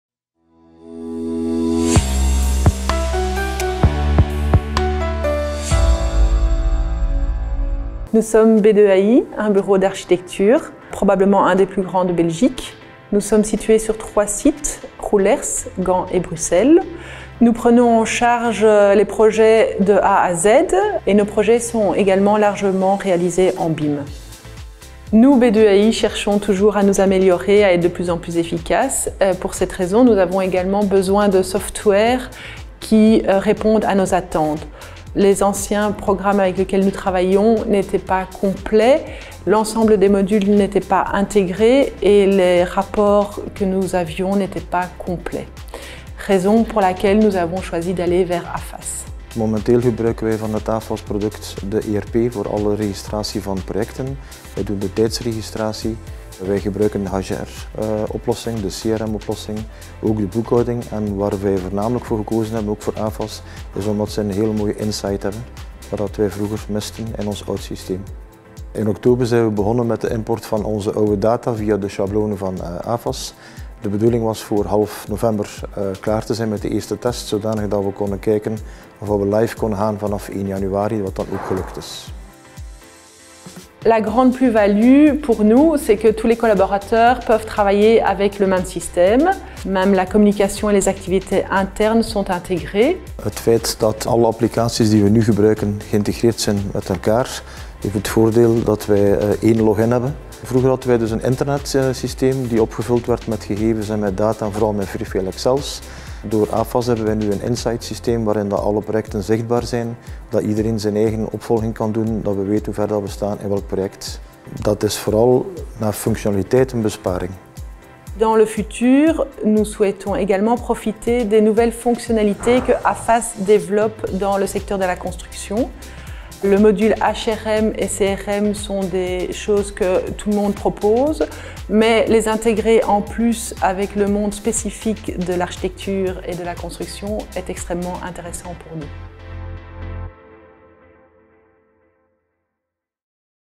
Klant van de Herfst: B2AI
Voor uitdagende projecten zijn ze bij B2AI niet bang. Sterker nog: de kracht van dit architectenbureau ligt in het bedenken en herdenken van grootschalige en complexe programma’s. Voor het beheer van hun vernieuwende oplossingen rekenen ze op AFAS. Daarom zetten we ze deze herfst graag extra in de kijker!